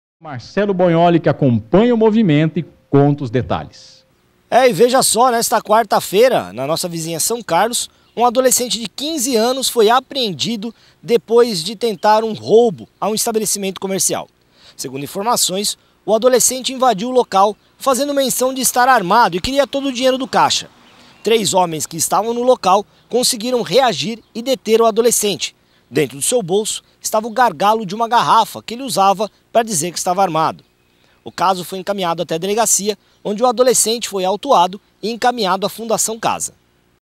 Adolescente é preso após tentativa de roubo em São Carlos
O autor usou um gargalo de garrafa para dizer que estava armado. Matéria veiculada no Jornal da Morada de Araraquara. Acesse nosso portal para mais notícias e reportagens: